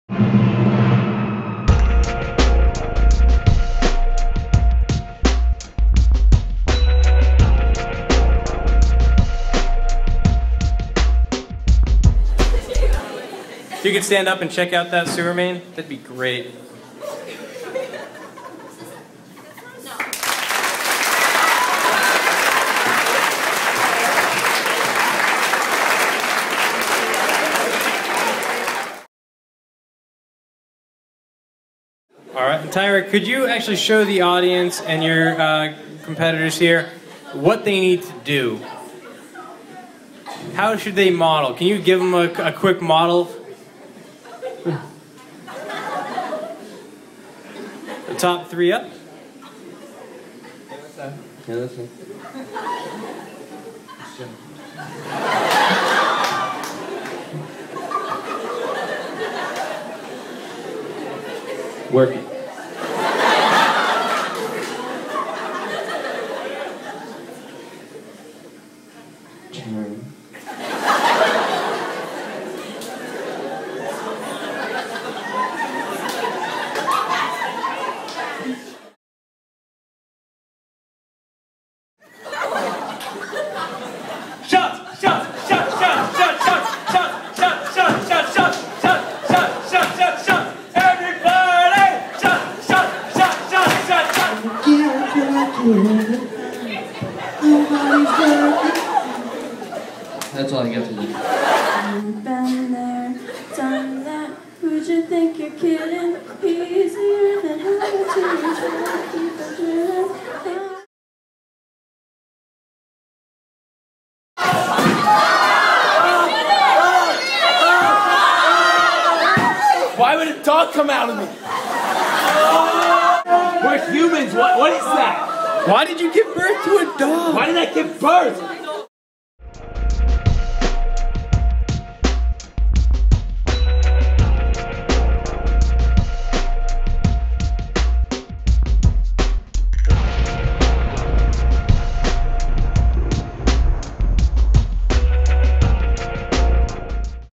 Comedy Hypnosis Skits from High School After Prom Party
Here are clips from a improv skit comedy hypnosis show performed by Rob Hackenson  during a High School after prom all night party!